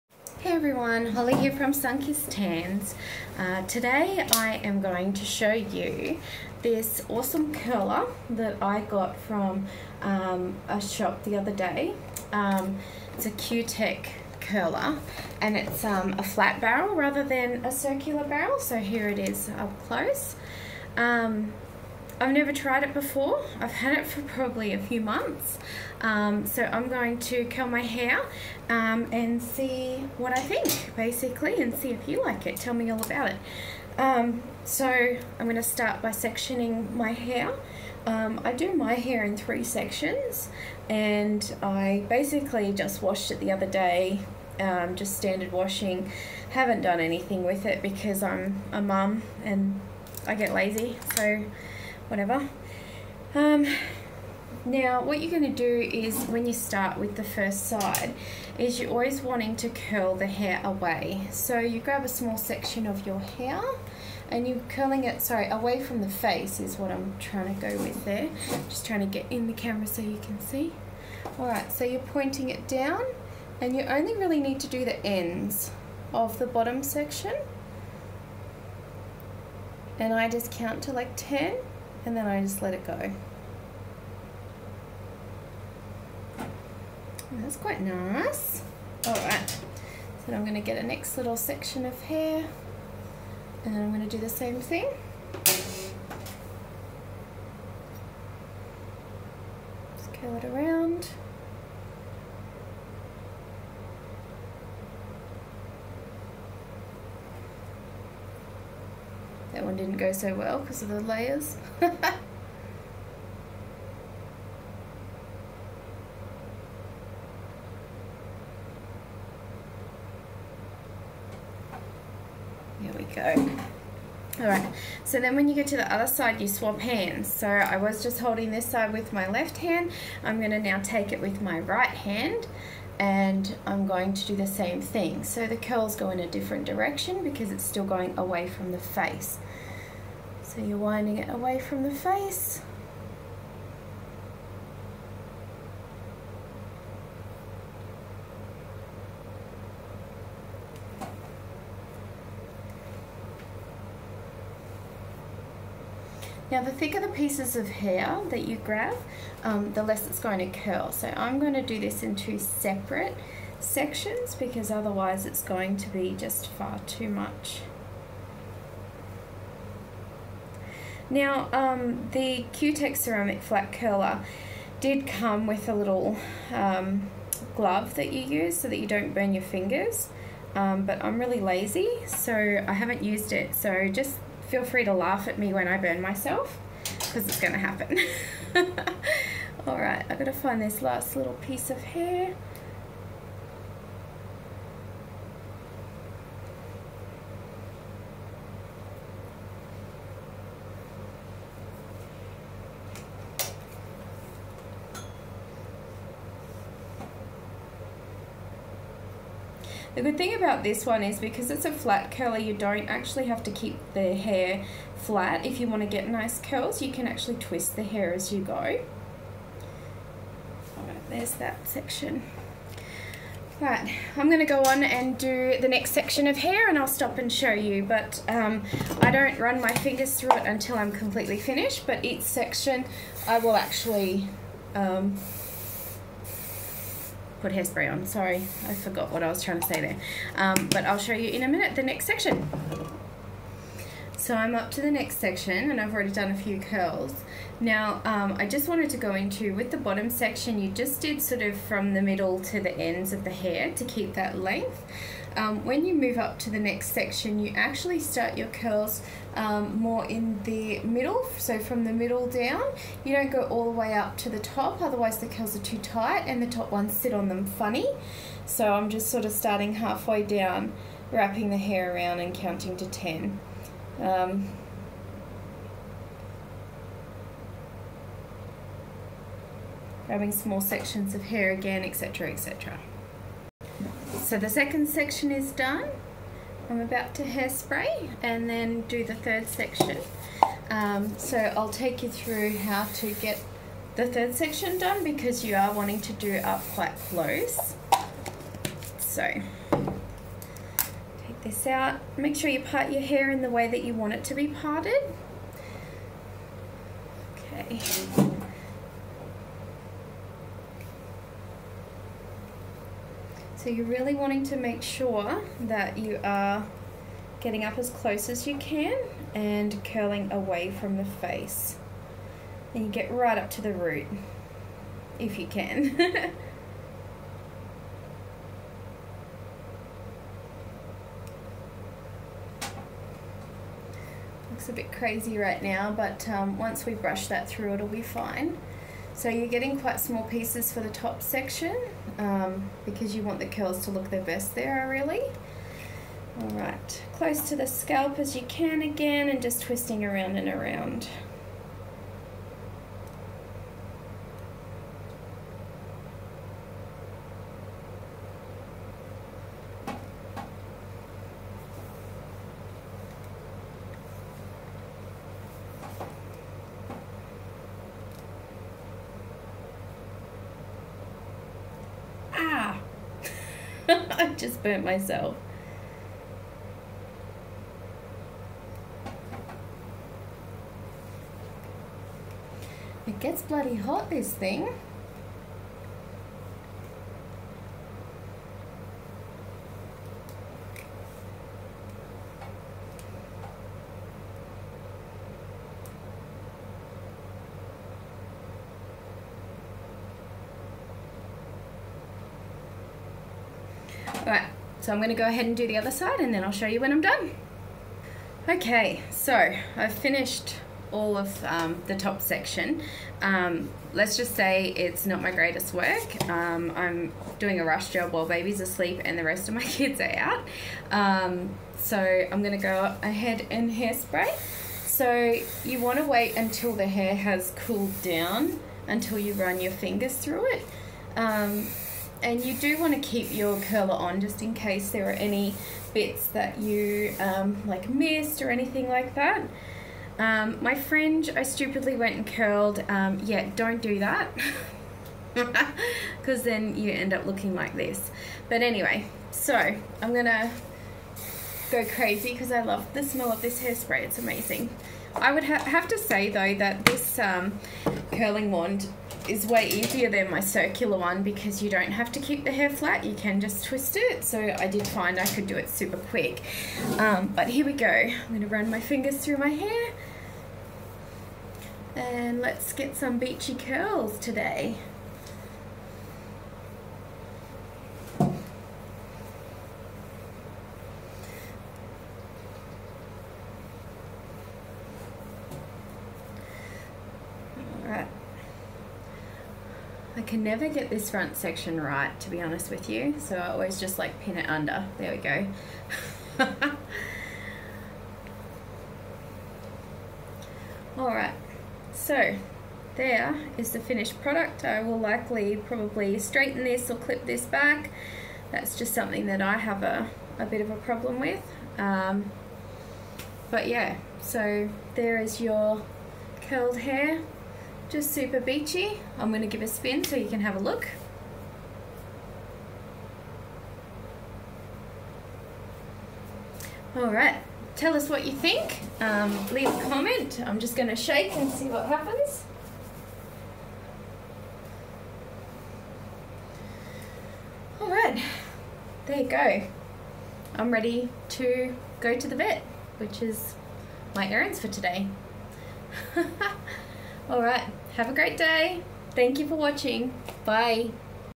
How to curl your hair using a Cutek curler
In this video we show you how to use a Cutek curler to give your hair beachy curls.